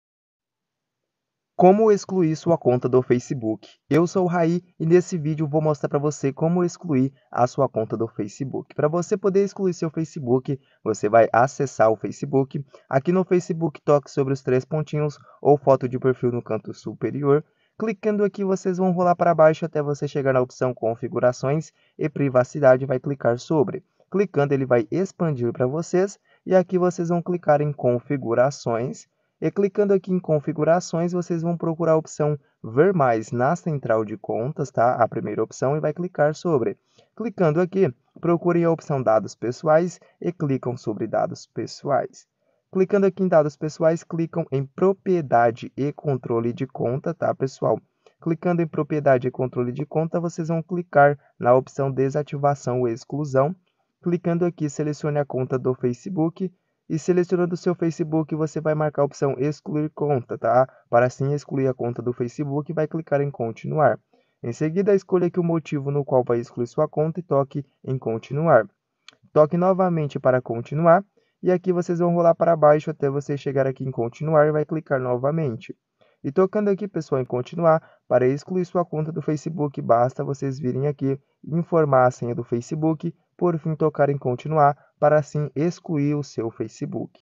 COMO EXCLUIR CONTA DO FACEBOOK em 2025
Saibam como excluir conta do Facebook, hoje neste vídeo vamos conferir como podemos excluir conta do Facebook, apagar conta do Faceboo é simples e fácil e rápido, assistam todo o vídeo de como excluir conta do Facebook.

Gratidão:)